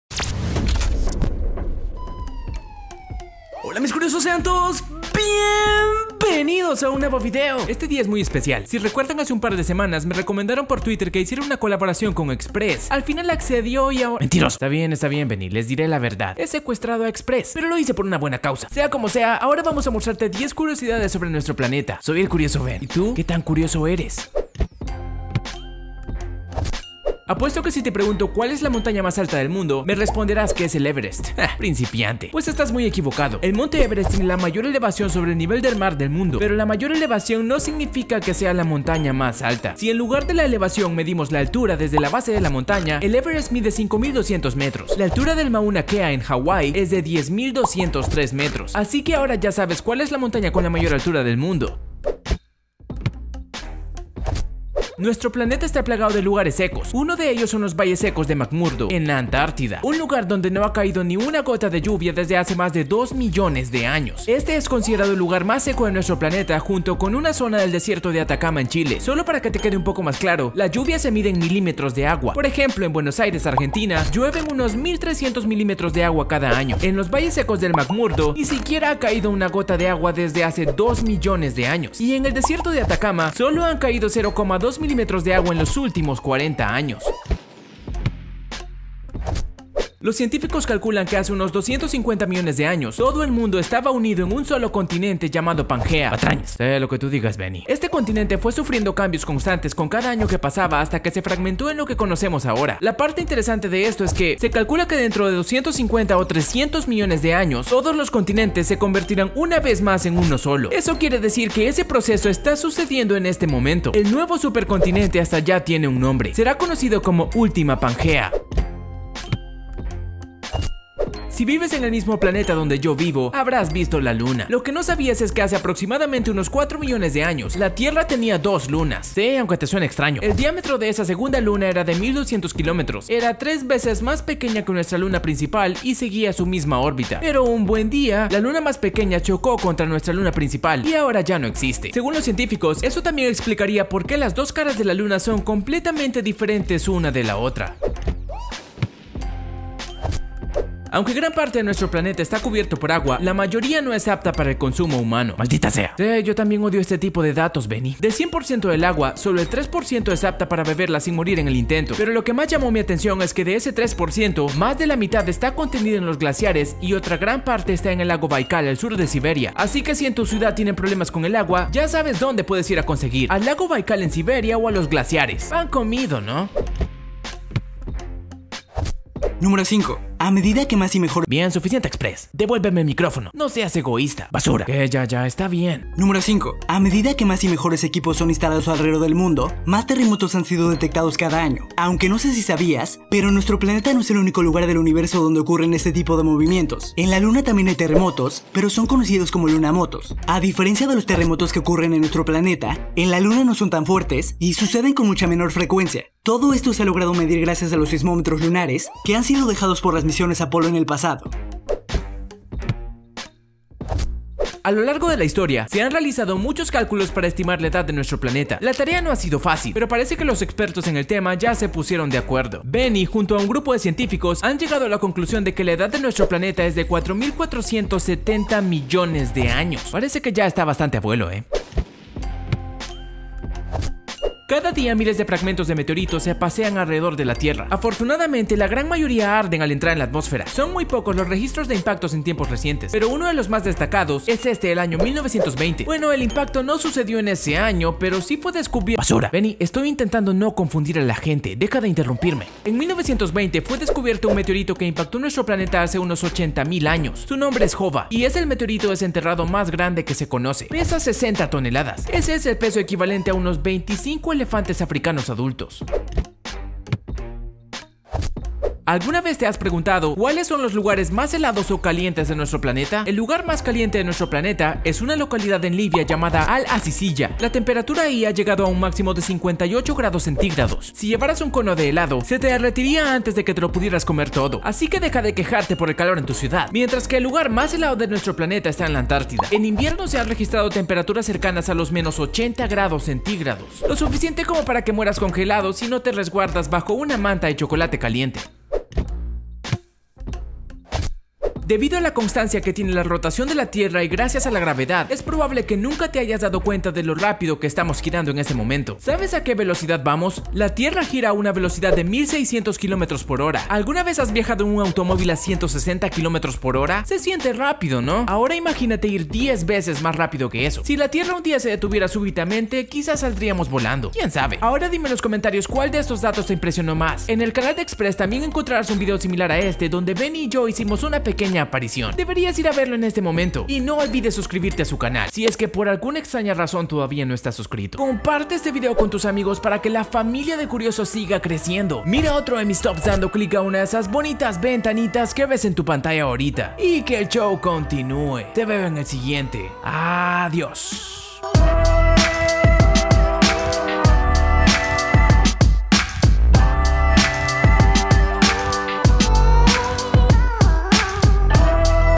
10 Curiosidades Del Planeta Tierra Con XpressTV
En este video podrás conocer 10 Curiosidades De La Tierra Con XpressTV (Colaboración con Xpress10)

– La montaña mas alta del mundo no es el Monte Everest, es Mauna Kea, en Hawai
– El lugar más seco de la tierra
– Dentro de 250 millones de años todo el mundo estará unido en un mismo continente: última pangea
– Hace aproximadamente unos 4 millones de años, la tierra tenía dos lunas.
– En la Luna también hay "terremotos", son conocidos como lunamotos.
– Del 100% del agua, solo el %3 es apta para beberla
– Benny junto a un grupo de científicos, han llegado a la conclusión de que la edad de nuestro planeta es de 4470 millones de años.
– En 1920 fue descubierto un meteorito que impactó nuestro planeta hace unos 80,000 años. Su nombre es Hoba y es el meteorito desenterrado más grande que se conoce
– ¿Alguna vez te has preguntado cuales son los puntos mas helados o calientes de nuestro planeta?
– ¿Sabes a qué velocidad gira la tierra?

Soy El Curioso Ben. Y tú, ¿qué tan curioso eres?

Ahora vamos a mostrarte 10 Curiosidades De La Tierra Con XpressTV.